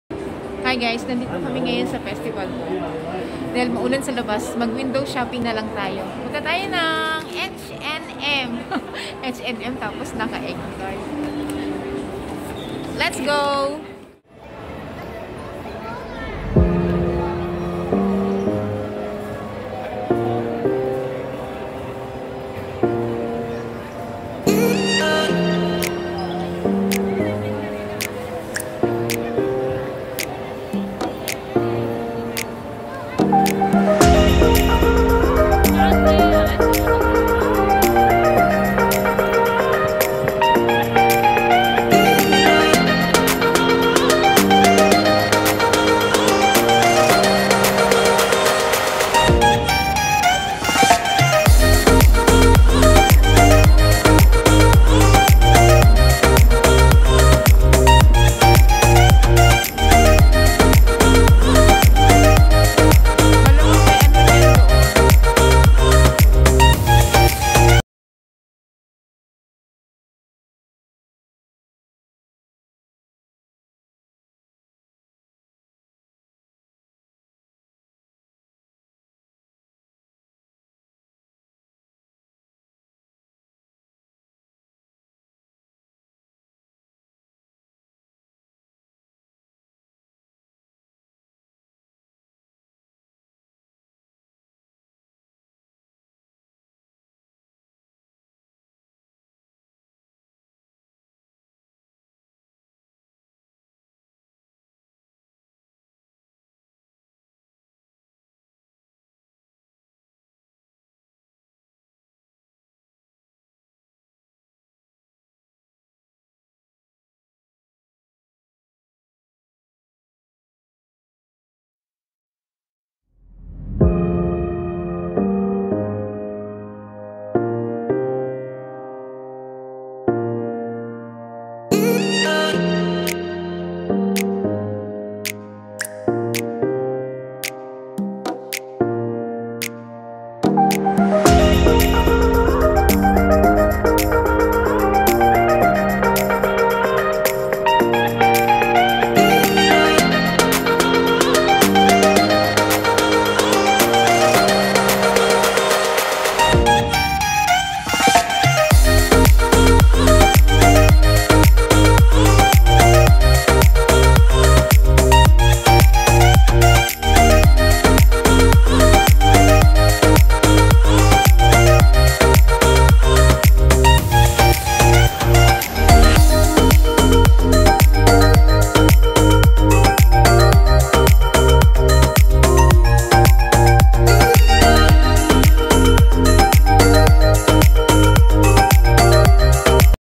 WINDOW SHOPPING AT H&M || YEAR OF THE TIGER THEME
nag window shopping  sa Festival walk dahil nasiraan ang train na sinakyan namin we decided na mag mall nalang so let's go guys hope you like this short  video and thank  you for  watching much appreciated godbless  all